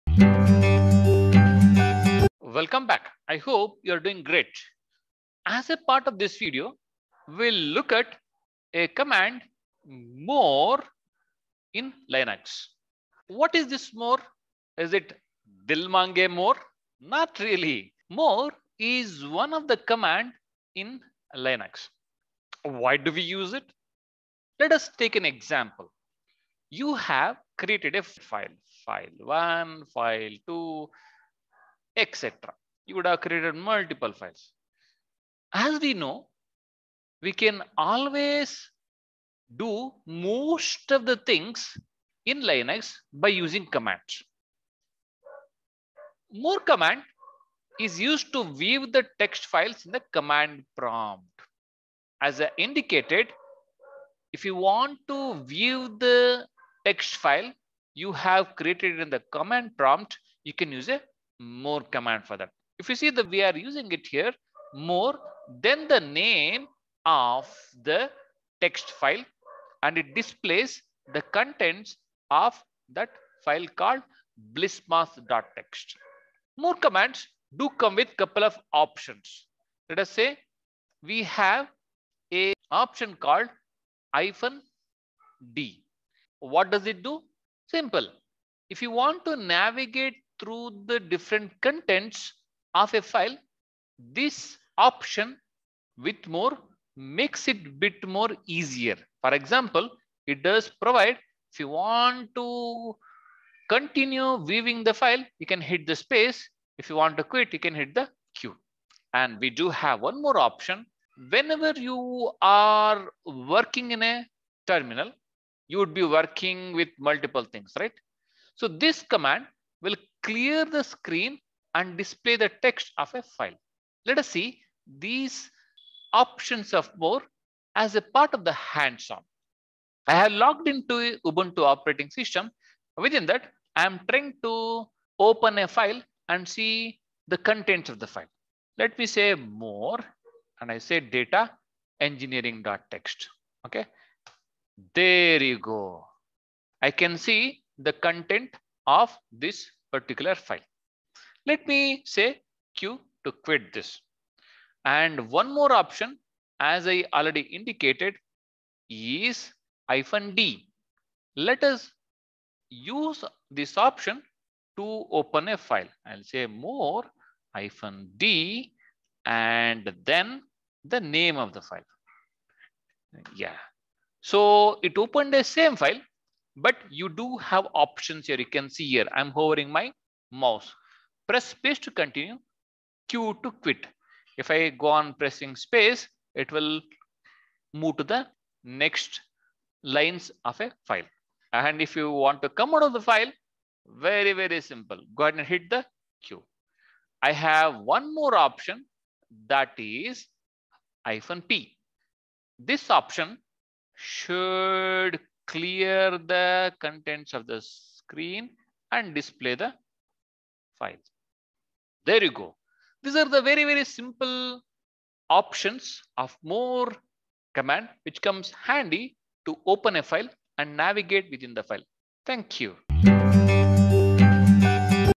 5. More Command in Linux
In this theory and hands-on video, you will understand More command in Linux.
More command is used to view the text files in the command prompt, displaying one screen at a time in case the file is large. It also allows the user do scroll up and down through the page.

If you have any questions about Linux? Ask us in the comment section below.